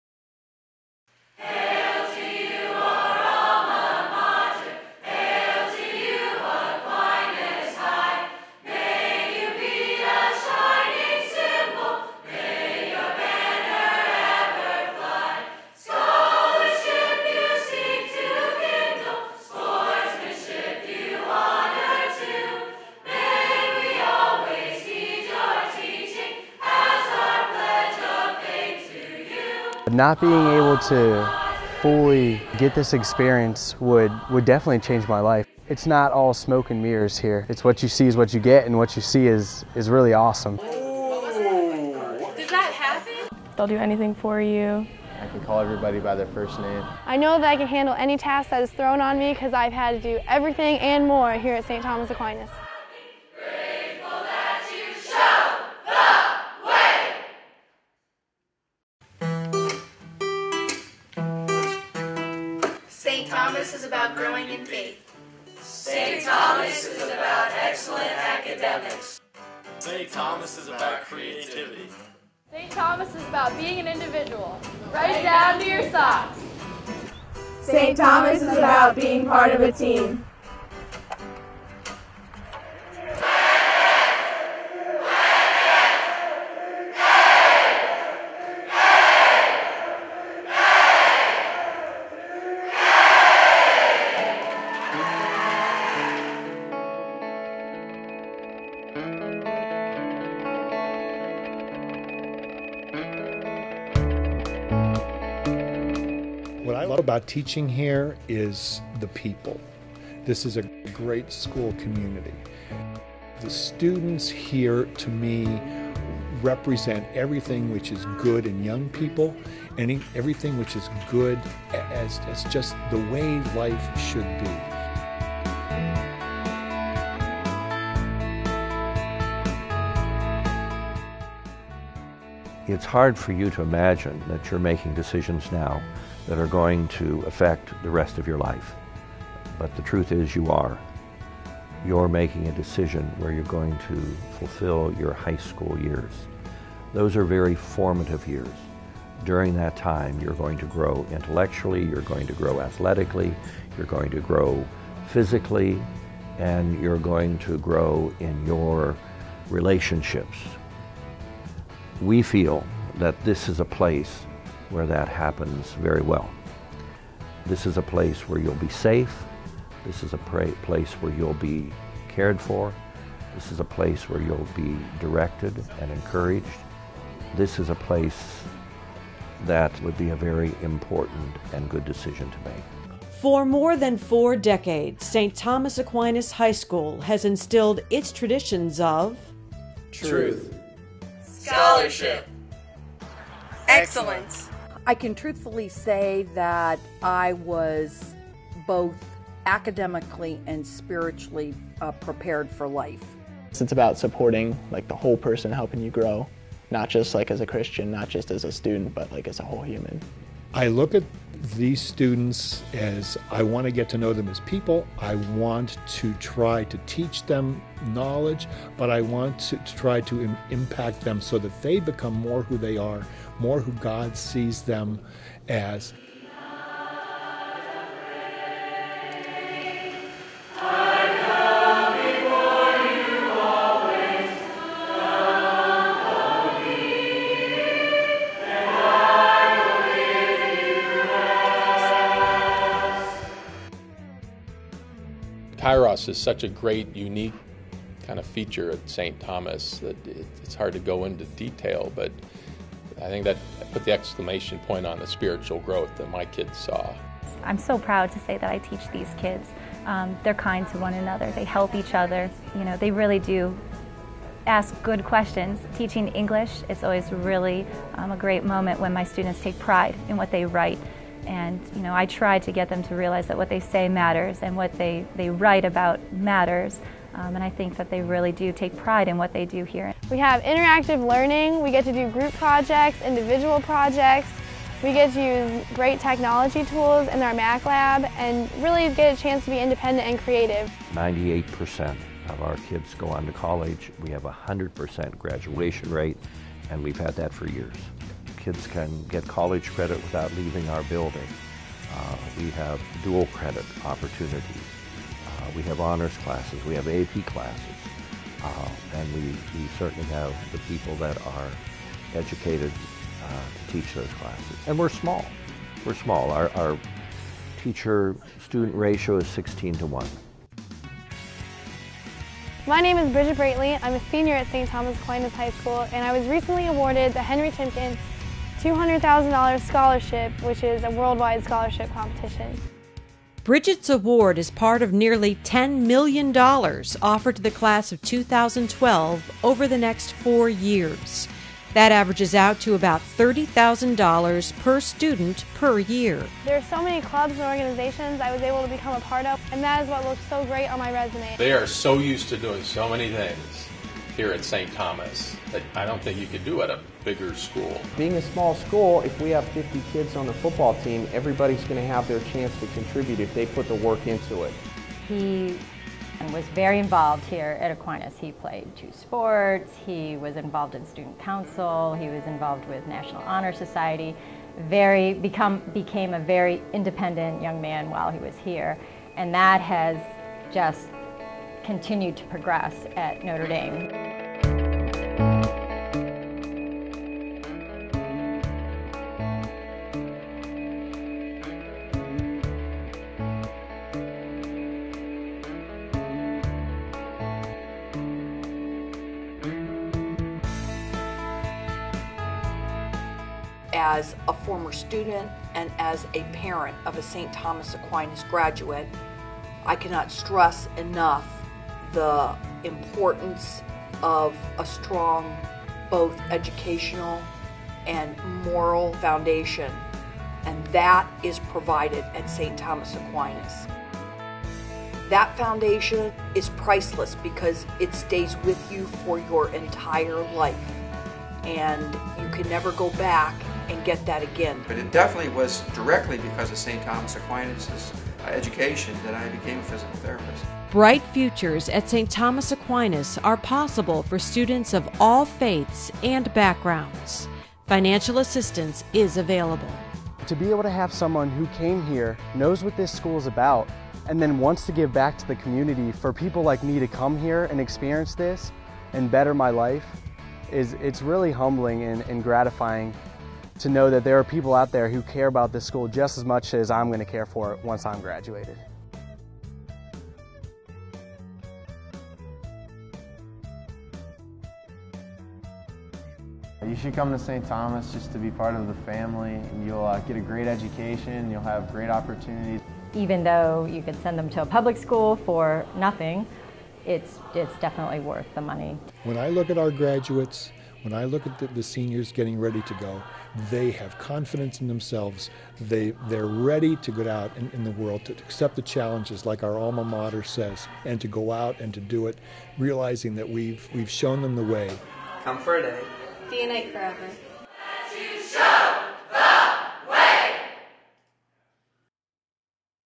St. Thomas Aquinas High School
St. Thomas Aquinas High School, located in Louisville, Ohio provides young men and women with a strong, Catholic, college-preparatory education to reach their full potential.  By engaging our students in mind, body and spirit, we help them become the leaders of tomorrow in Stark County and the world. 98% of our students go onto college and 100% graduate. Come see what the Aquinas difference is all about.